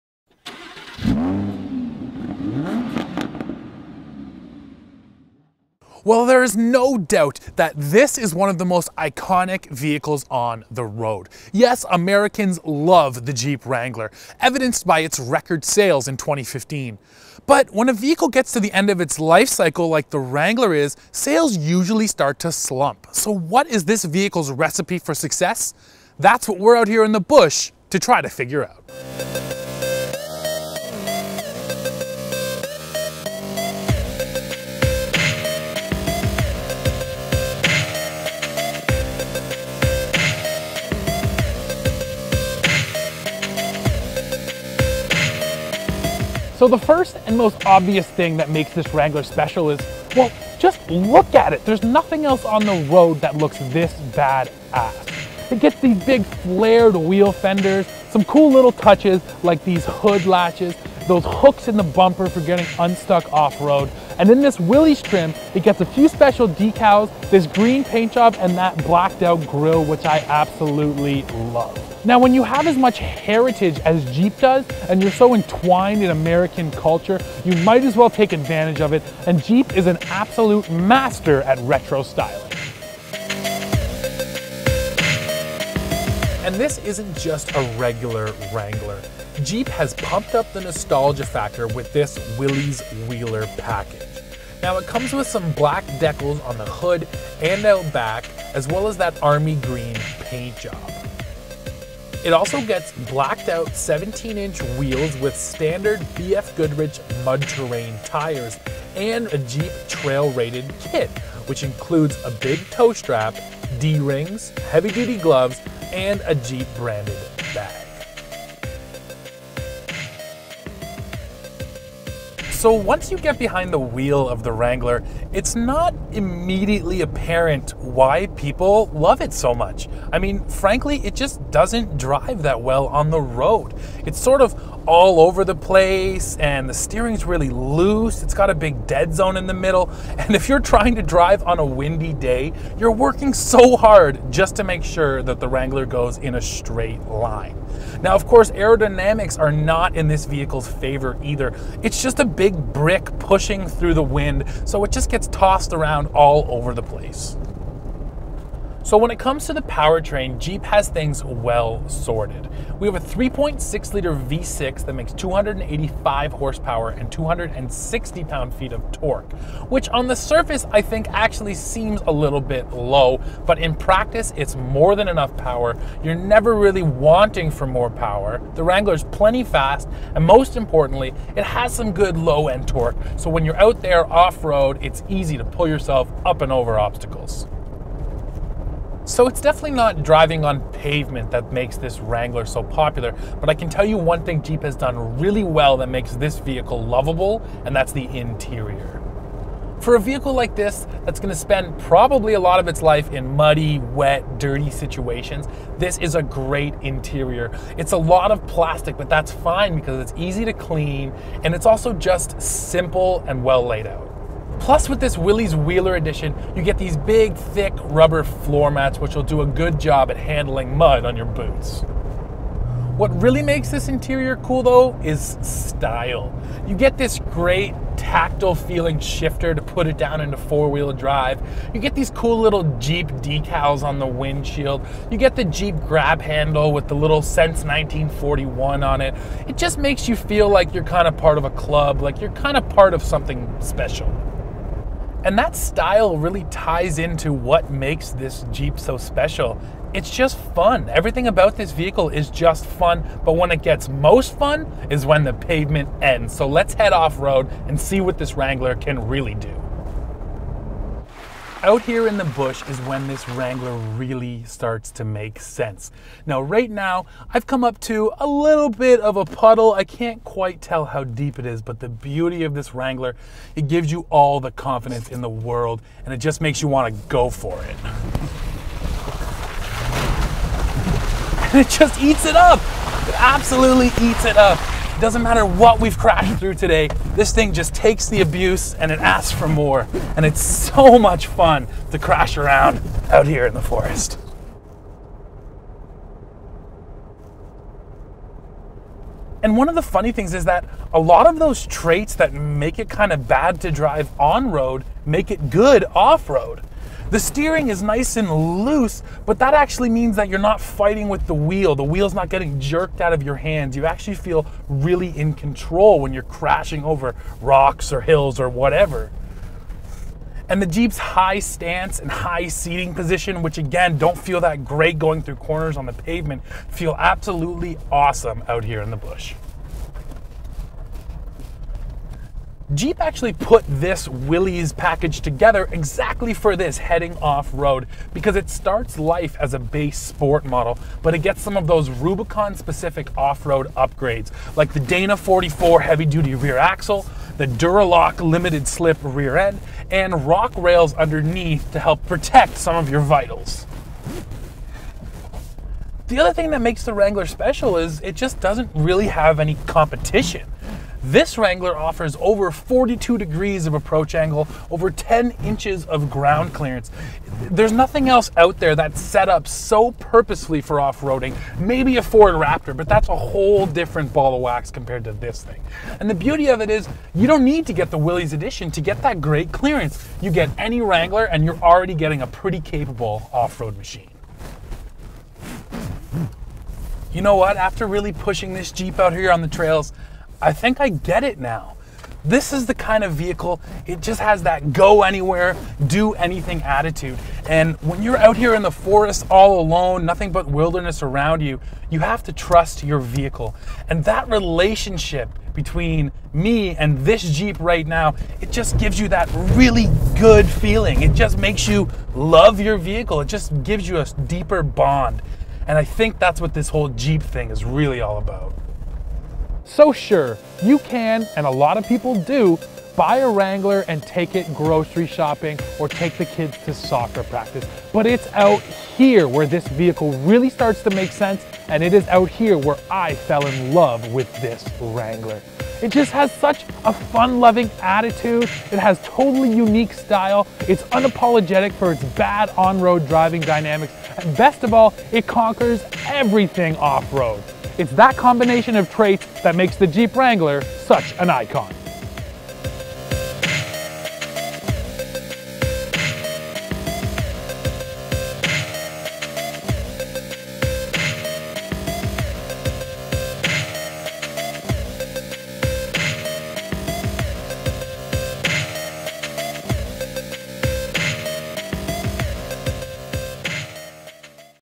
2016 Willys Jeep Wrangler Review
What makes the Willys Jeep Wrangler such an icon? We take it off-roading to find out.